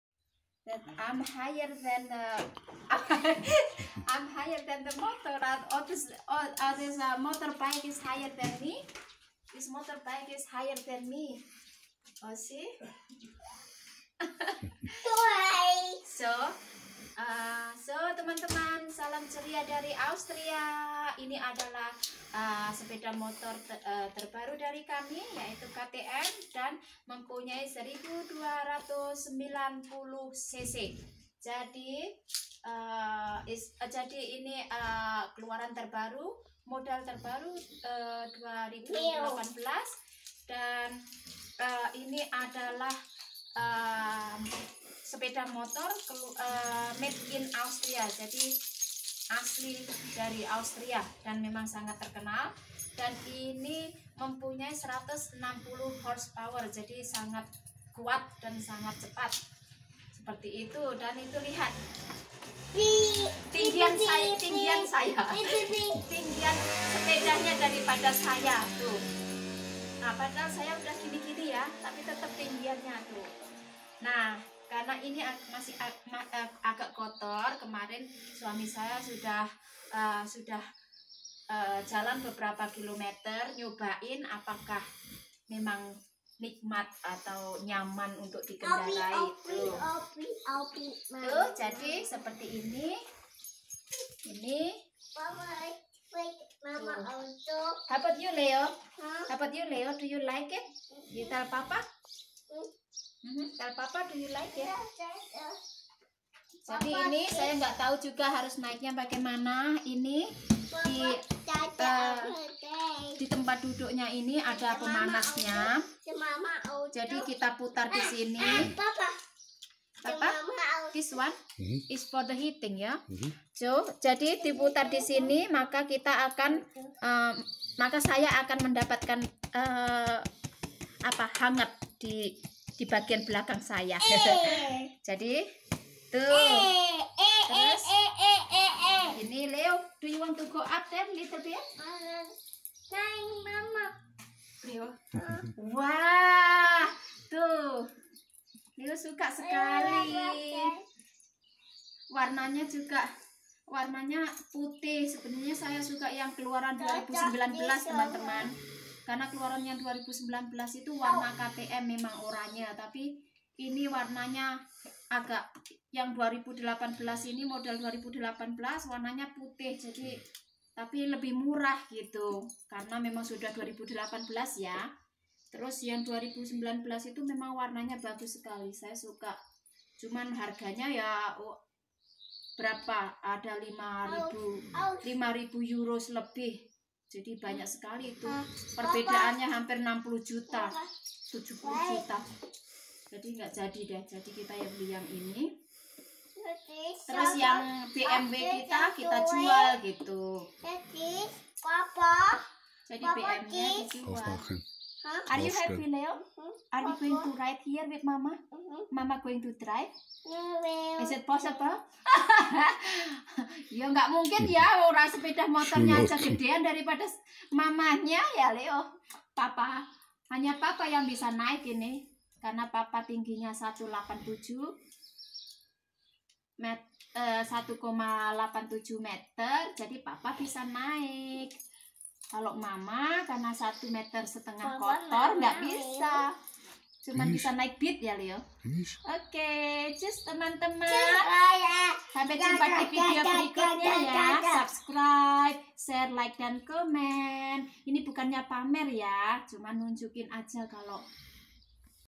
SUAMI BULE BELI MOTOR GEDE BARU || review sepeda motor KTM buatan Austria
Hai teman-teman semua. Salam ceria dari Austria 🇦🇹 Eropa. Review sepeda motor KTM 1290 CC Super Adventure-S 2018 model ,vlog dalam Bahasa Indonesia.